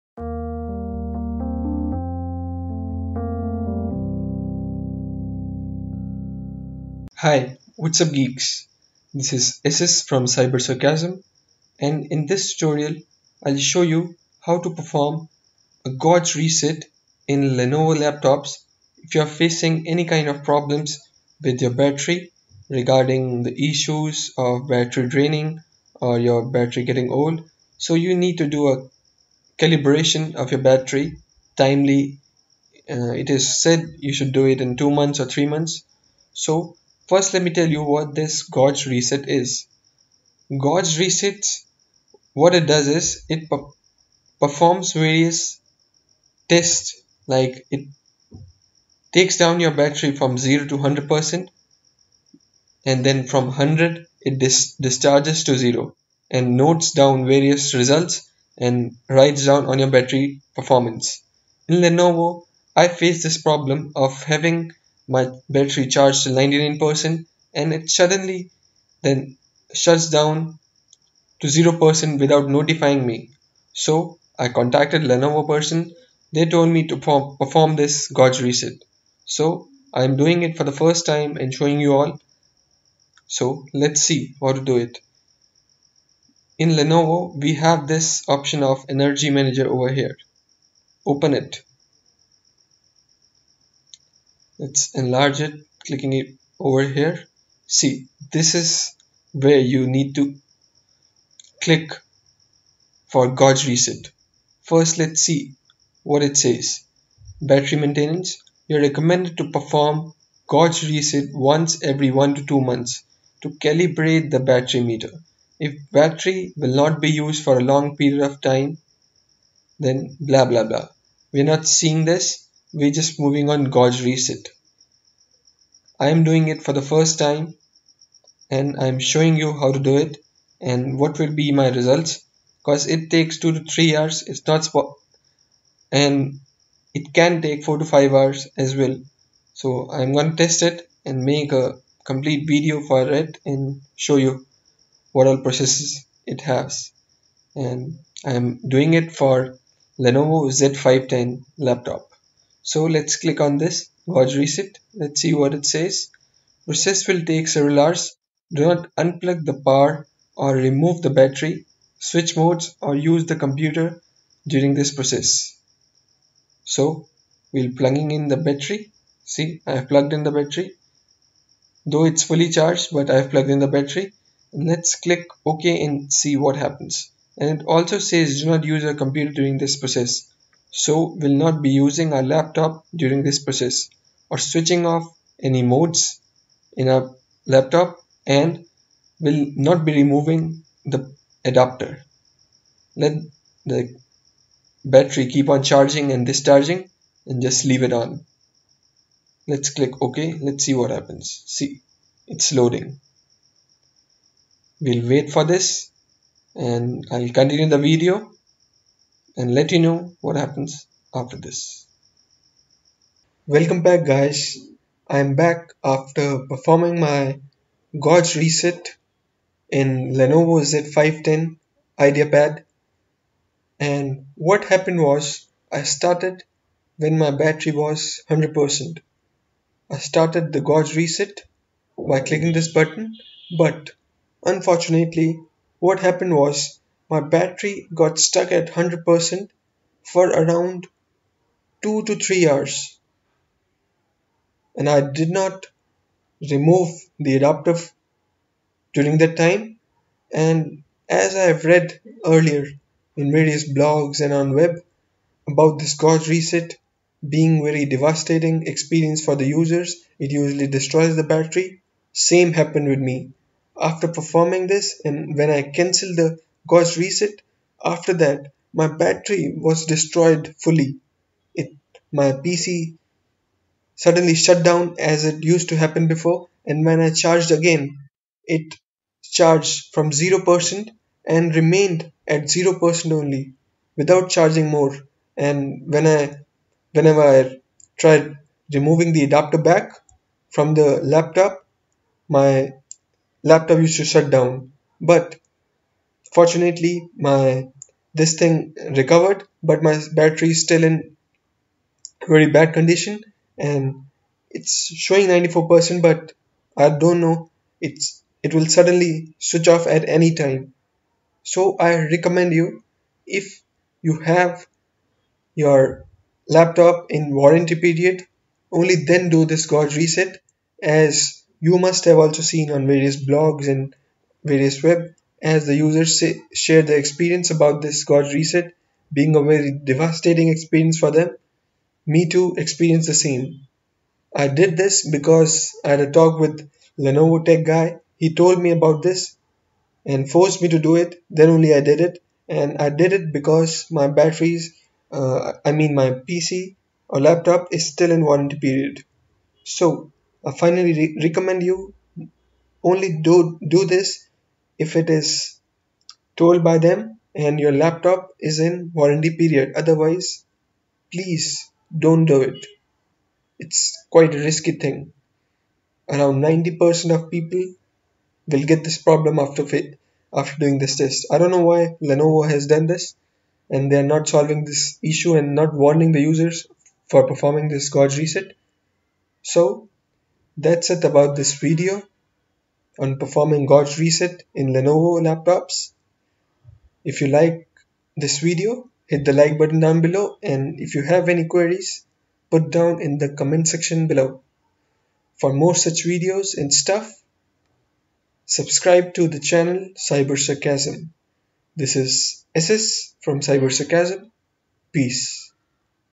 How To Perform A Gauge Reset In Lenovo Ideapad Laptops
Learn How To Do Gauge Reset In Lenovo Ideapad Z|Y Series Laptops